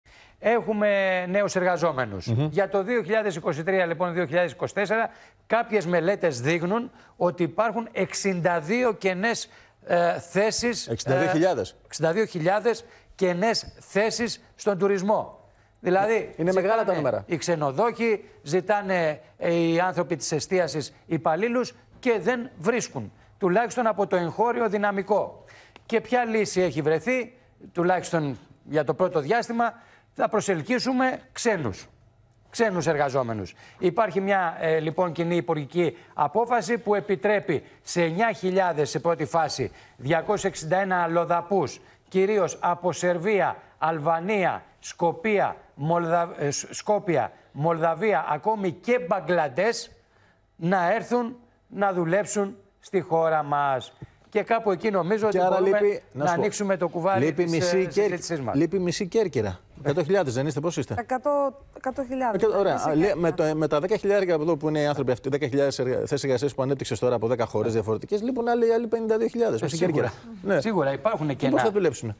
Αρνούνται να δουλέψουν σε τουριστικές επιχειρήσεις οι νέοι – Στις 62.000 οι κενές θέσεις | OPEN TV
Οι επιχειρήσεις του τουρισμού δανείζονται εργαζόμενους από το εξωτερικό για το καλοκαίρι.